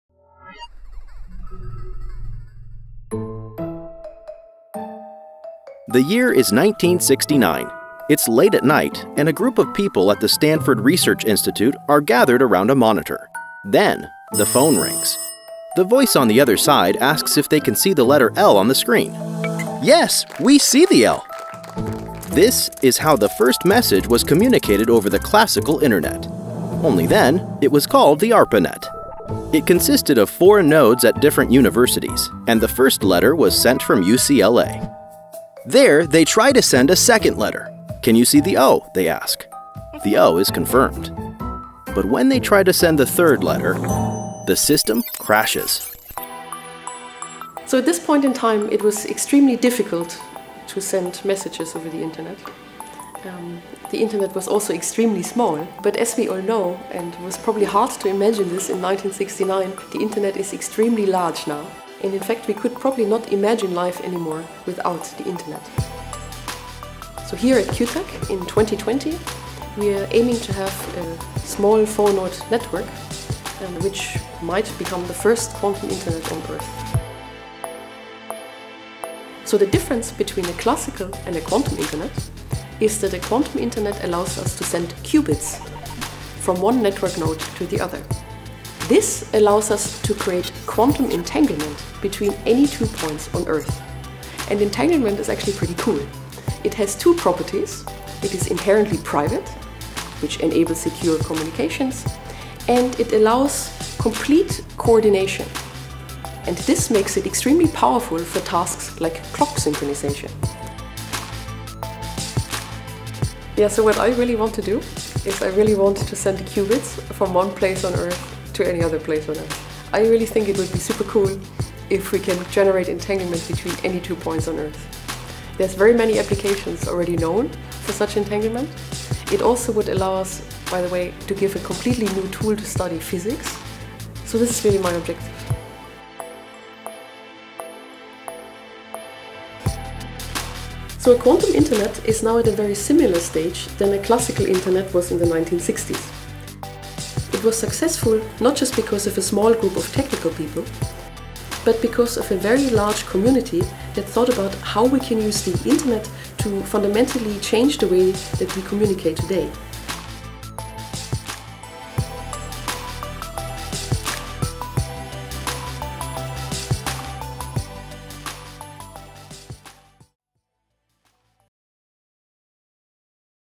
Introduction to the Quantum Internet | DelftX on edX
There is no doubt that quantum computers and the quantum internet will have a great impact on our world. But we don't yet know quite how. As with traditional computers - we will only see the effects in the decades to come.

This course will provide you with a basic understanding of quantum computing and the quantum internet. Together, we'll peek into the fascinating world of quantum phenomena, such as qubits, superposition, and entanglement.